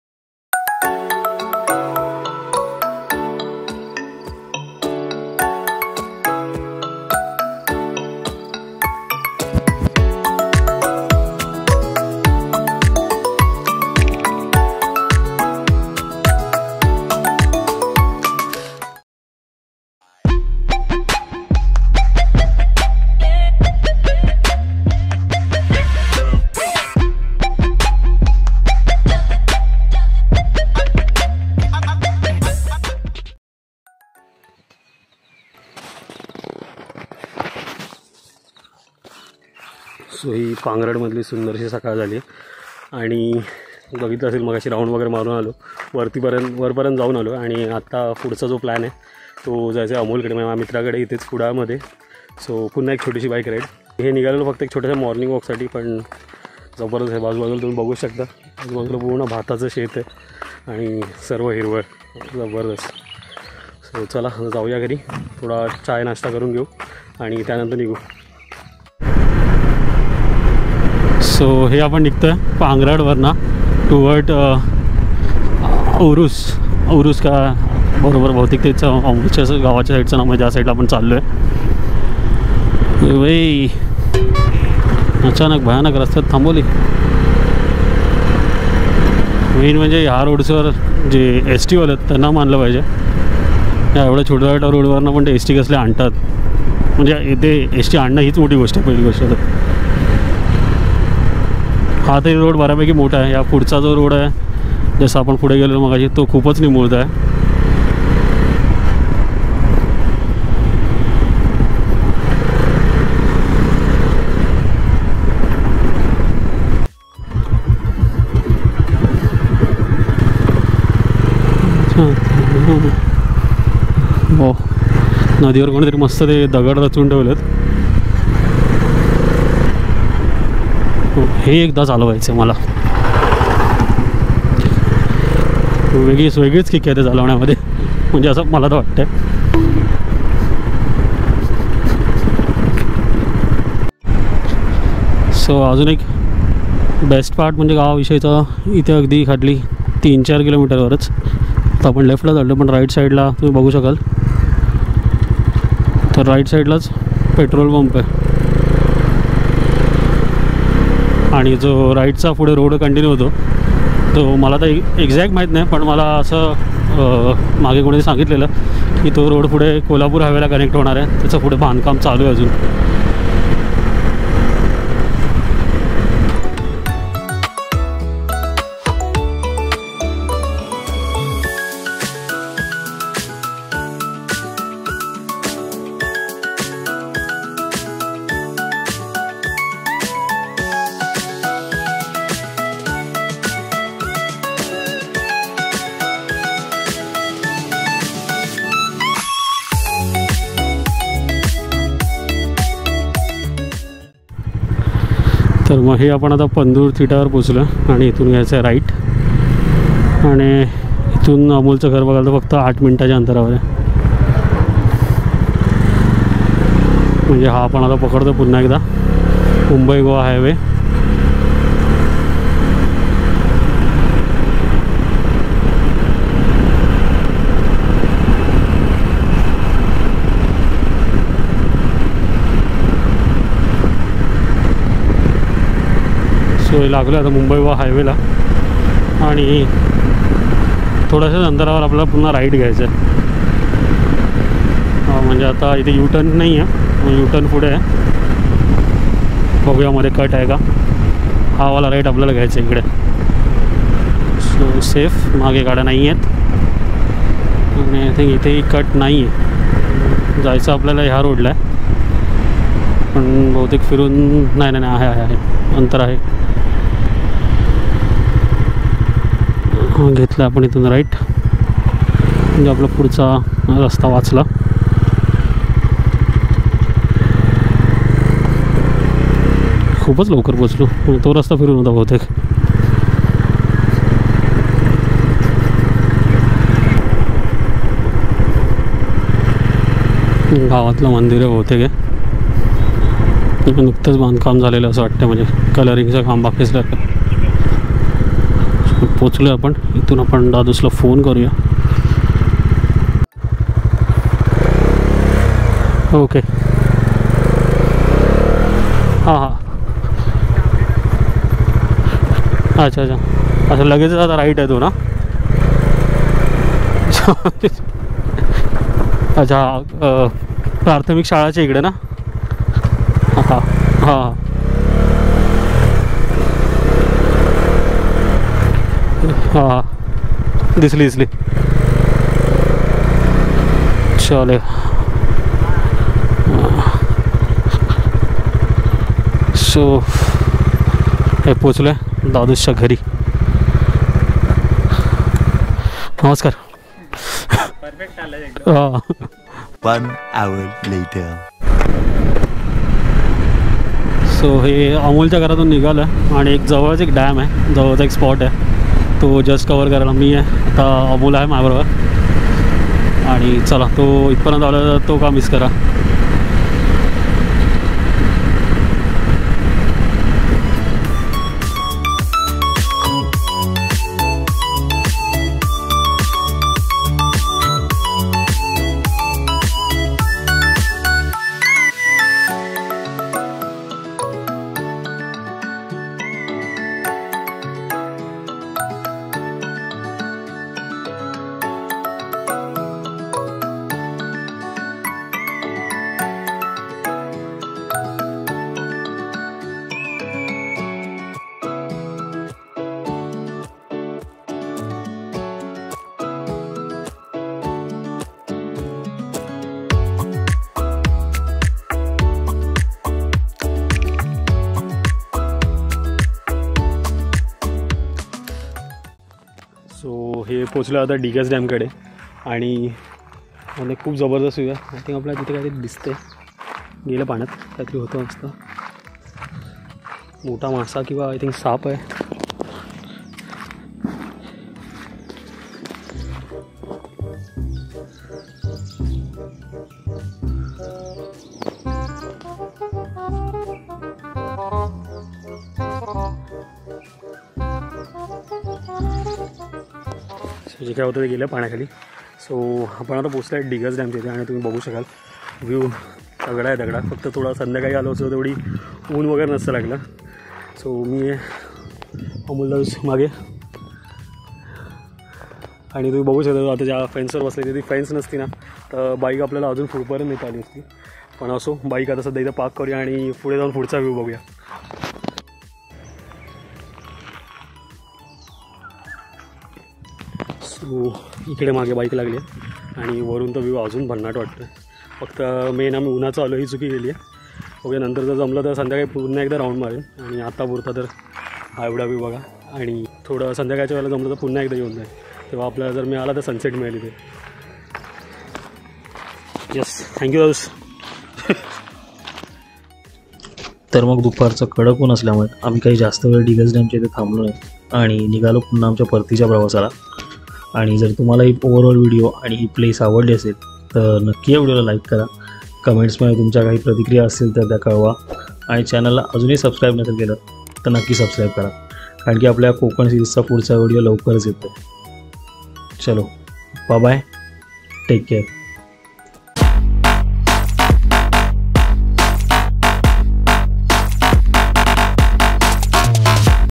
Digas Dam - Kudal | Bike ride | Dominar 400 | Marathi vlog | EP - 4
Presenting you forth episode of my series Konkan Bike ride - Ganpati Special
I started my ride from Mumbai. Exploring Satara, Kolhapur, Amboli , Kumbral, Kudal, Pangrad and returning via Pune - Bangalore Highway.

Stay tune for the upcoming Konkan episodes.
I will be uploading new episode each week from now so stay tune.

Motorcycle I used = Bajaj Dominar 400UG
Vlogging Camera = Gopro Hero 7 Black
Helmet = SOL
Riding Jacket = RR Gear
Gloves = Pro-Rider
Saddle Bag = Viaterra - Condor
Top Box = ASG 45L

Thanks!